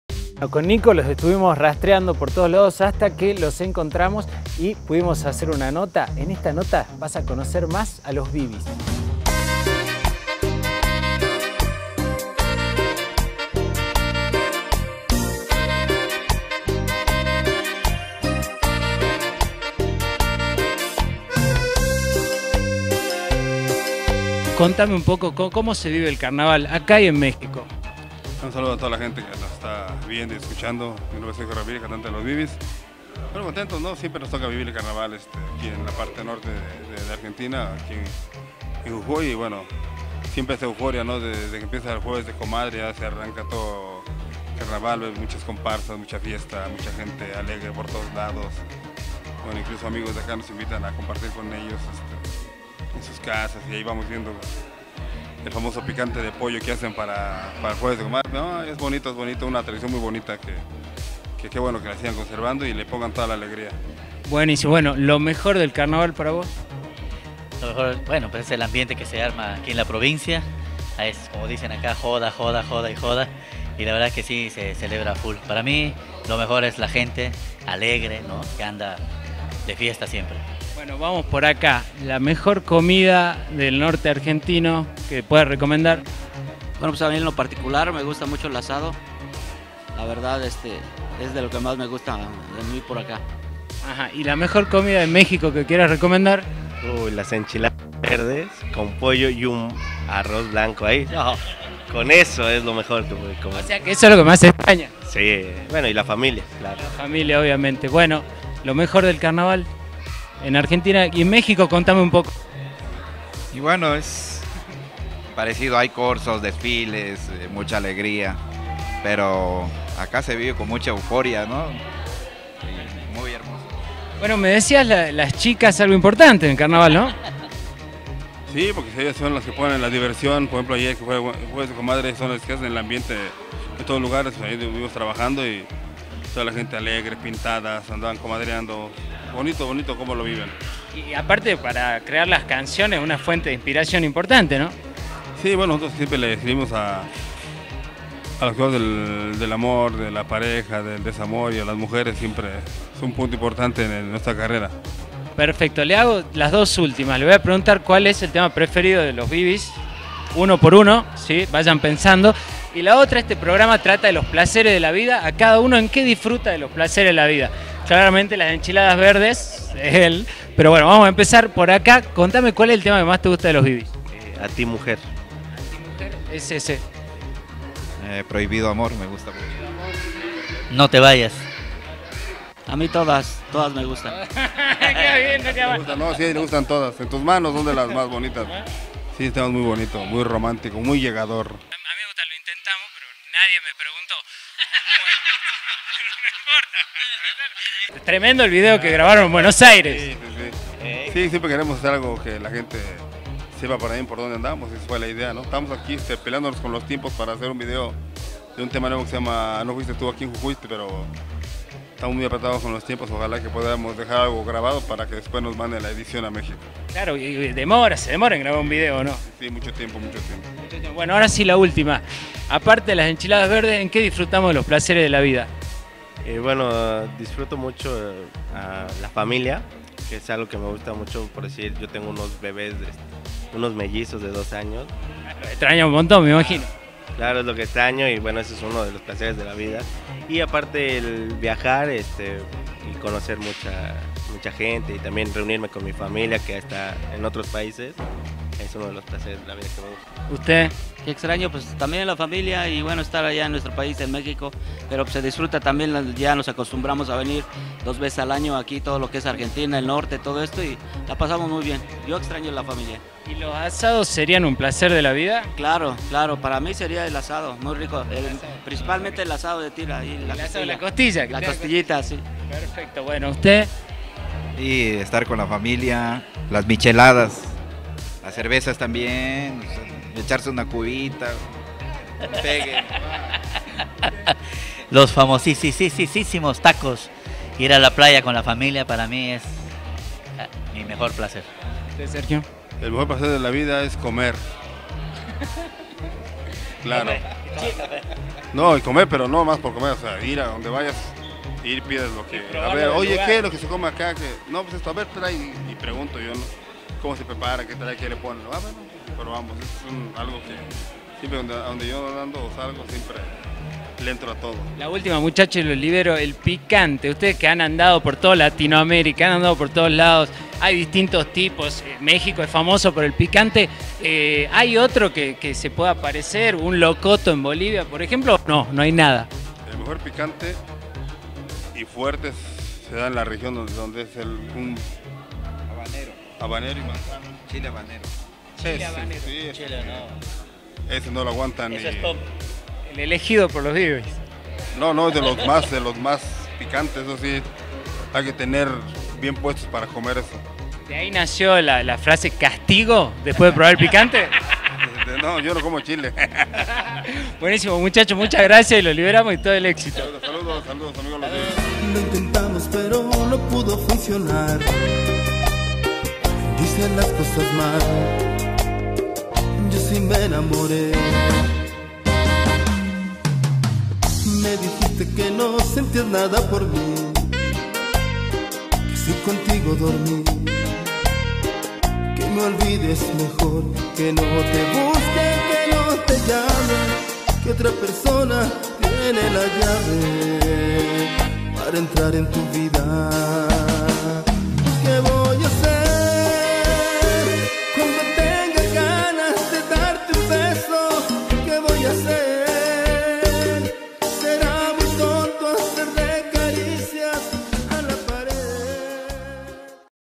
Los Bybys en el carnaval de Jujuy
En pleno carnaval, mientras recibían sus remeras intervenidas por Juan Manuel Taritolay Gius. para tocar en el escenario de LOS TEKIS OFICIAL. Logramos encontrar a los reyes de la cumbia romantica en Jujuy. Conocimos mas a los liderados por Los Bybys - Sergio Ramírez miralo en ésta nota de la mano de Pedro Soler Los BYBYS "en Argentina" Los Bybys Mx
camaras y edición Nico Cordeyro Nicolás Cordeyro - Diseño Audiovisual
look Humahuá